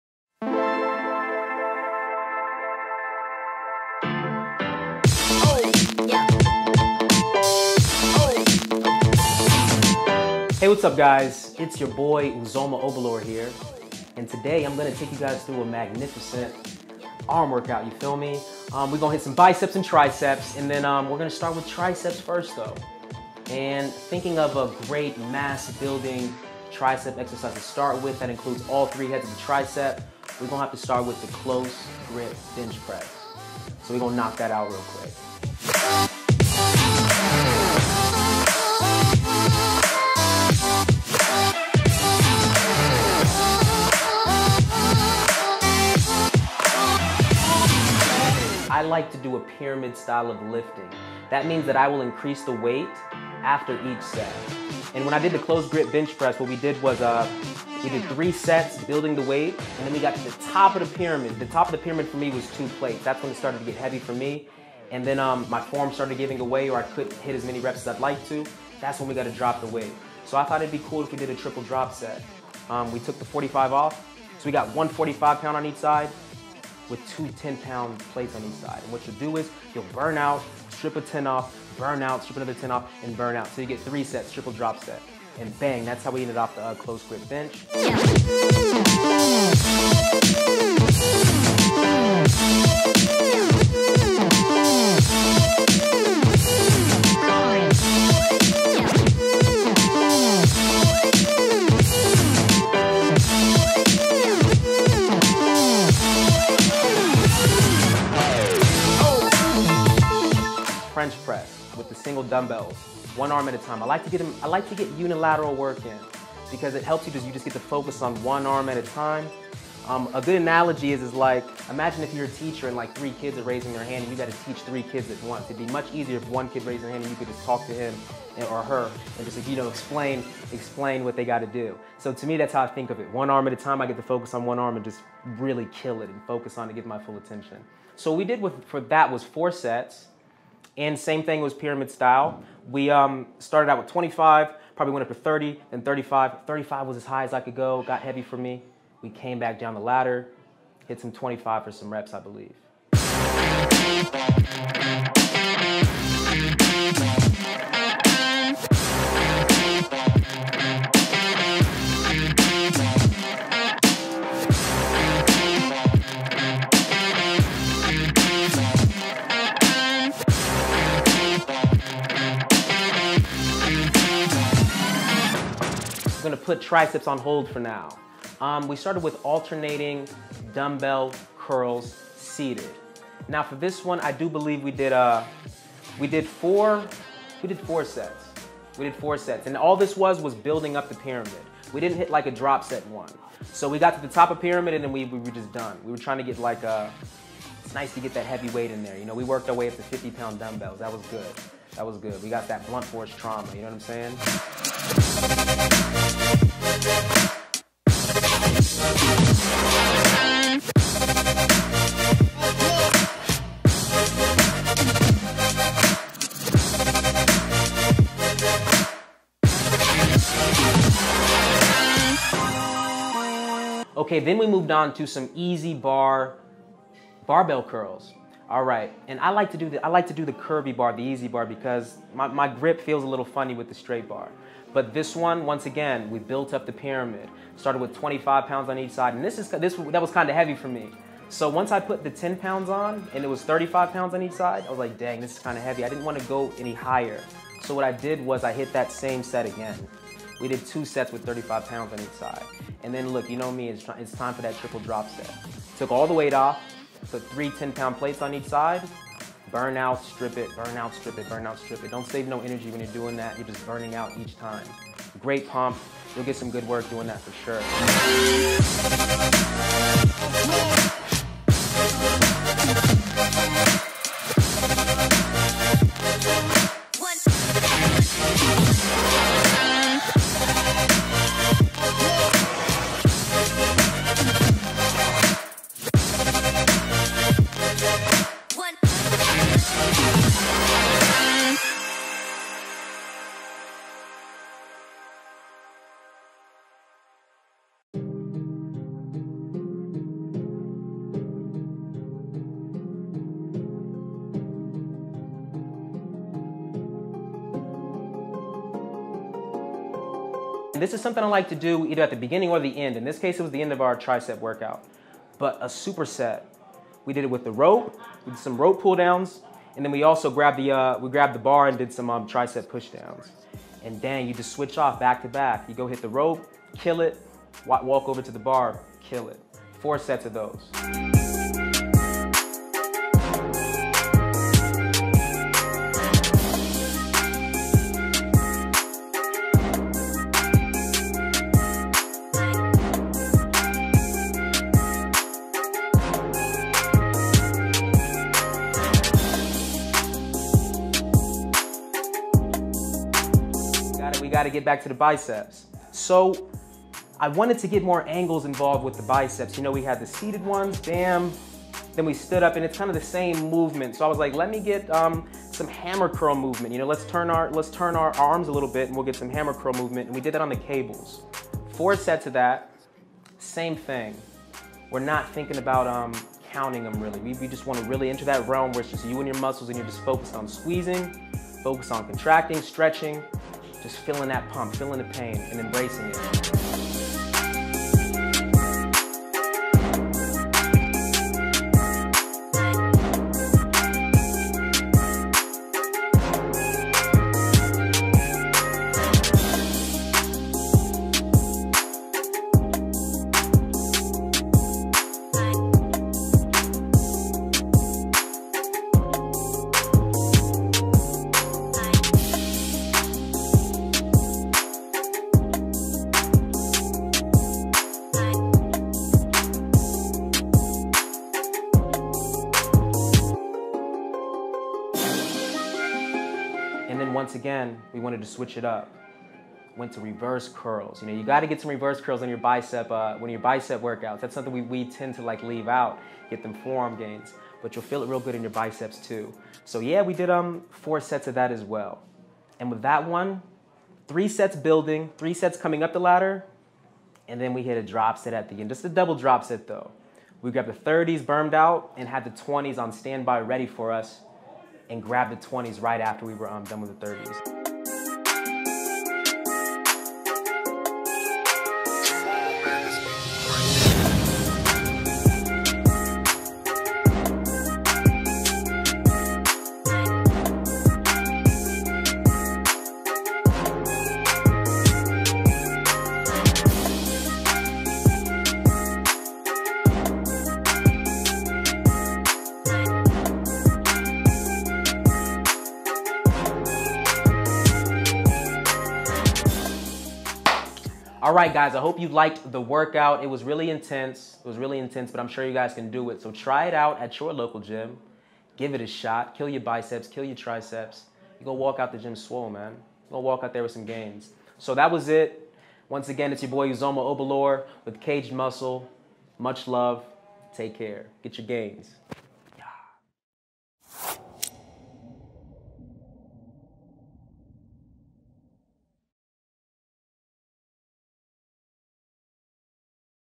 Full Biceps & Triceps Workout for Bigger Arms | Uzoma Obilor
Think you know how build bigger arms? Kaged Muscle athlete Uzoma Obilor shares his full arm workout to help you add some size to your biceps and triceps.

If your arms are your major goal right now, don't be afraid to put most other body parts on maintenance level. Still train them, but don't murder them. You want to be able to give your arms all the energy you can!

| Uzoma Obilor's Arm Day Workout |
1. Close-Grip Bench Press: 3 sets, 12-15 reps
2. Dumbbell One-arm French Press: 4 sets, 8-12 reps
3. Seated Alternating Dumbbell Curls: 4 sets, 15 reps
4. EZ-Bar Curls: 5 sets, 6-10 reps
5. Superset
    a. Rope Triceps Pressdown: 4 sets, 12-15 reps
    b. Straight-bar Triceps Pressdown: 4 sets, 12-15 reps
6. Cable Hammer Curls: 4 sets, 10-15 reps
7. Dumbbell Reverse Curls: 4 sets, 6-12 reps